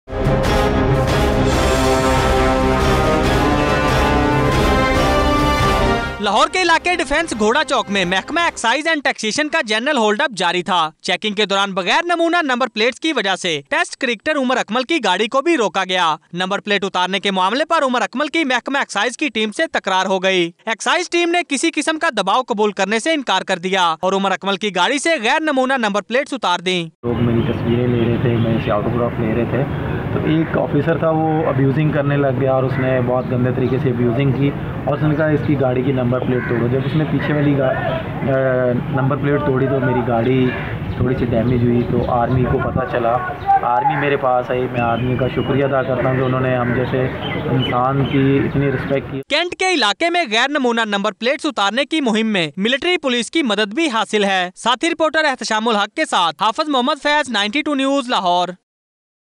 Umar Akmal struck in another controversy over fancy number plate 02-03-2017 - 92NewsHDPlus
Watch News Report : Umar Akmal struck in another controversy over fancy number plate ,  92NewsHDPlus